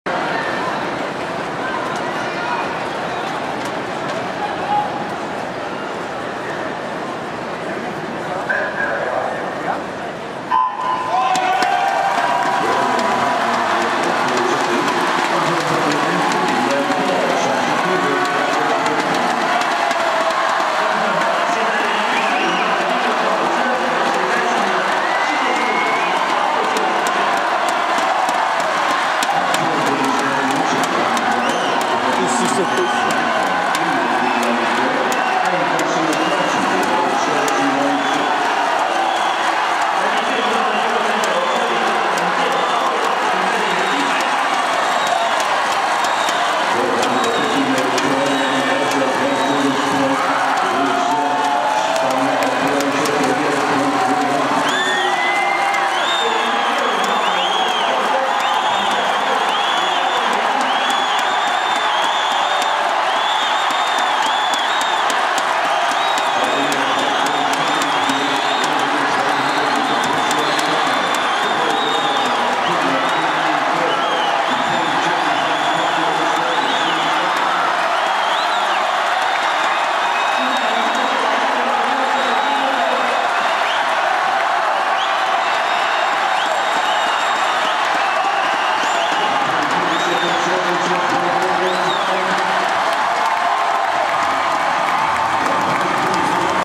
Nastup Mihovila Španje na 100 metara prsno
Nastup Mihovila Španje u disciplini 100 metara prsno  staza 1